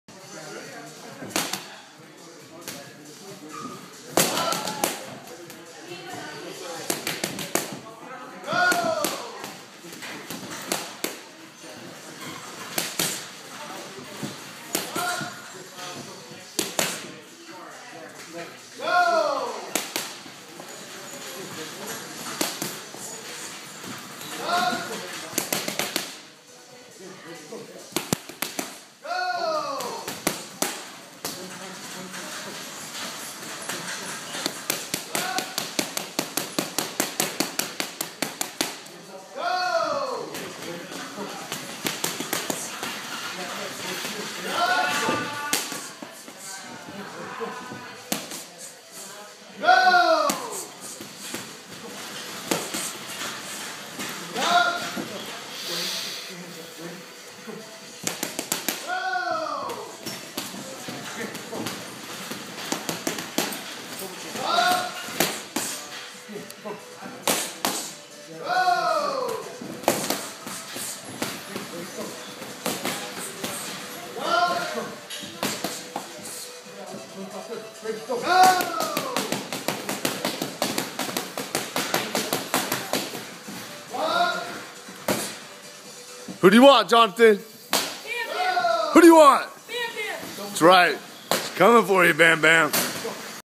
Pretty Boy Bam Bam Challenge!!!!
I Johnathan Rubio challenge u pretty boy bam bam to a match I'm with double punches boxing club in Northern California it has to be USA boxing show.  Just for fun of it. You better train hard!!!